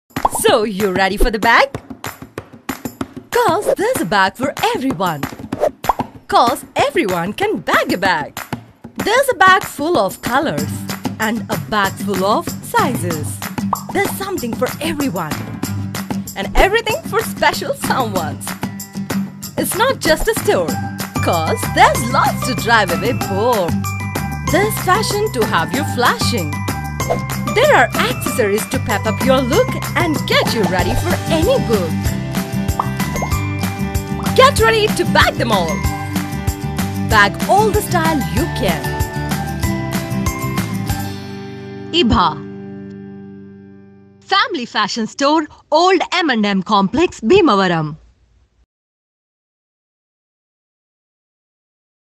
Ibha Promotional Video by White Thoughts and Bradning
We created a promotional video for Ibha, a family fashion store in Bhimvaram, Andhra Pradesh. We took the brand's core message of complete family shopping and revolved it around the concept of bagging it.

The call-to-action of 'BAG IT' captivated and emotionally connected to the audience, thus creating a buzz which helped them associate with the brand.